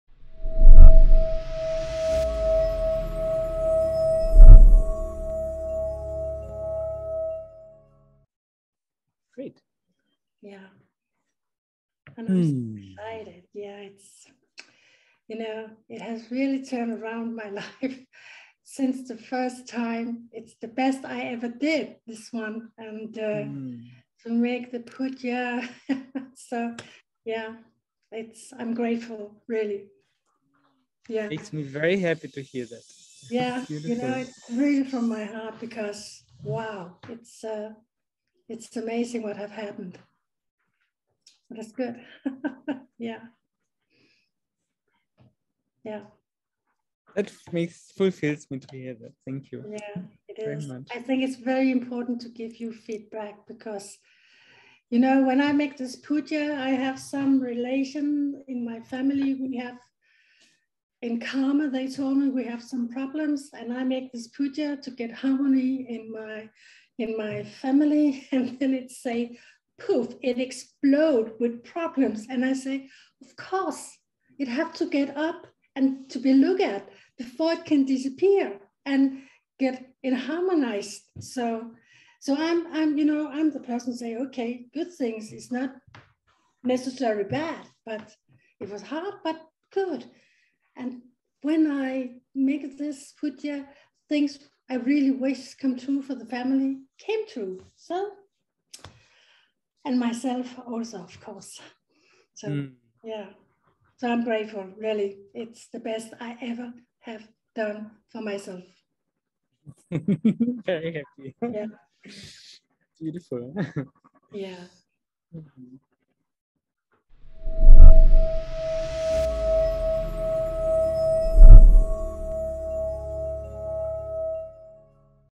How the effects of the palm leaf remedies can look like
This is a short excerpt from a conversation with Sophia, our Danish translator of the palm leaf readings. We had this conversation before she had a chapter reading, which is why it was recorded and we though it is worth sharing.
She shares about the effects of the Pujas, which were part of the palm leaf remedies. The remedies are recommendations that are given in the readings on how ee can transform karmic blockages.

If you feel a calling to find your palm leaf, you are very welcome